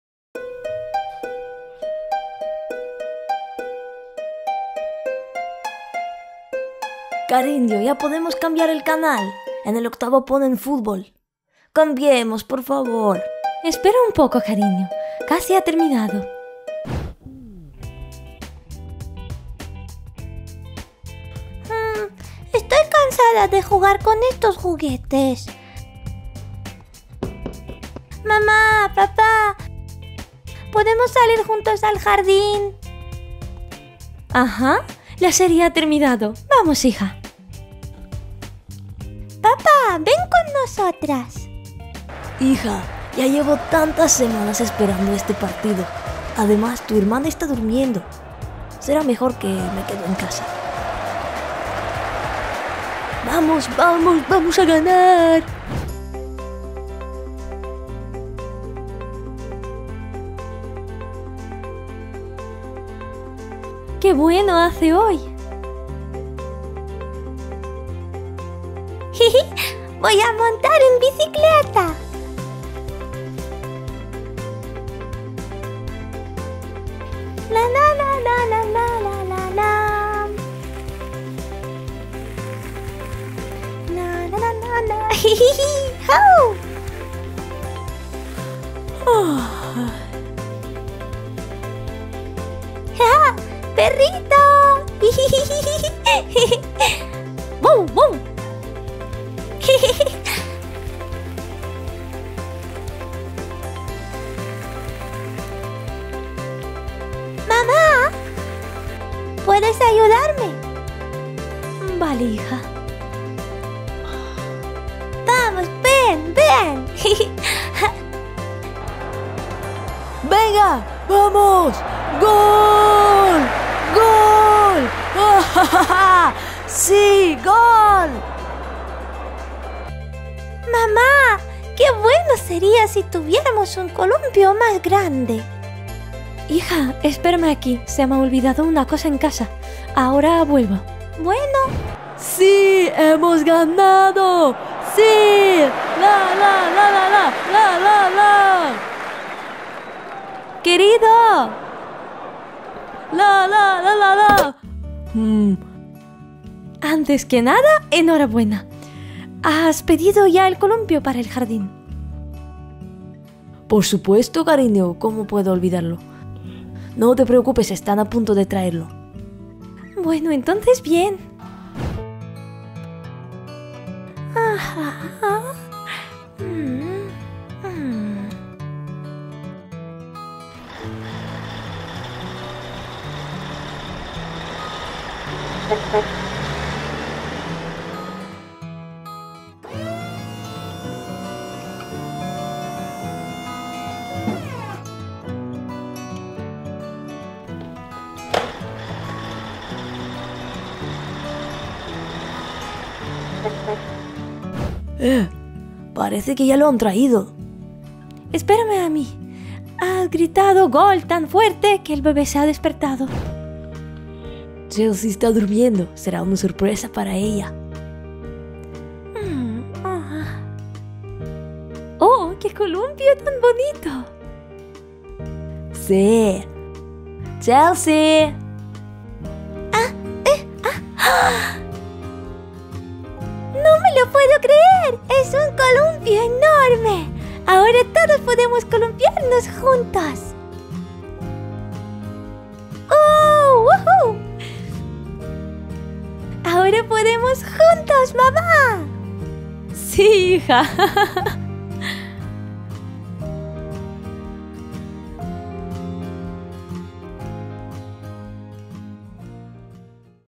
Barbie muñecas descansan en jardín. Vídeos para niñas.
Vídeos para niñas de Barbie muñecas. Hoy hace muy buen tiempo. Barbie y su hijas quieren descansar en el jardín. Papá Ken prefiere quedarse en casa y ver un partido de fútbol. Barbie, Chelsey y pequeña chica van al jardín. Chelsey está montando la bicicleta, Barbie y la niña están tumbándose en la hamaca. ¡Qué día maravilloso! Pero sería mejor si tuvieron un columpio grande para toda la familia. ¡Vamos a comprarlo!

👶¿Qué está haciendo pequeña Bianca?